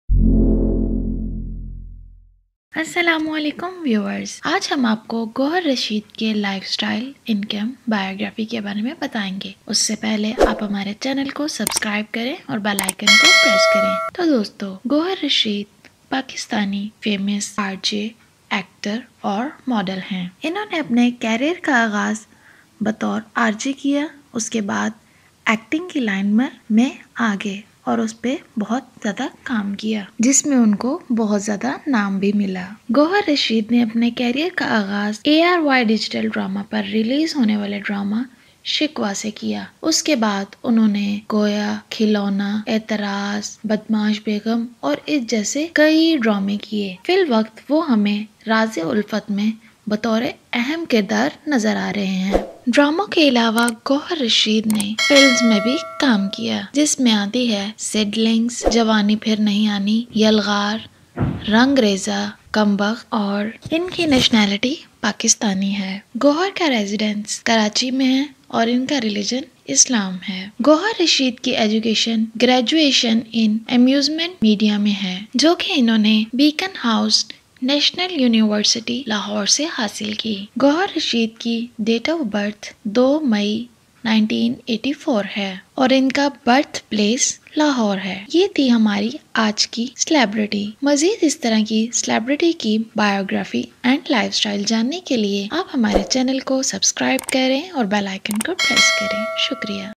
GOHAR RASHEED Biography, Age, Education, Wife, Family, Children, Drama List And Movies
Gohar Rasheed an impressive and versatile actor, Model and a radio representative. He was conceived in Lahore on 2nd May 1984 and is a grandson of famous Islamic scholar Ghulam Ahmed Perwez.

Gohar Rasheed got his initial school instruction from GC University, Lahore. He got his college degree with majors in theatre, film, TV, and minors in journalism from Beaconhouse National University. He further on did his graduation in amusement media from Beaconhouse National University, Lahore.
you can read more about this Drama on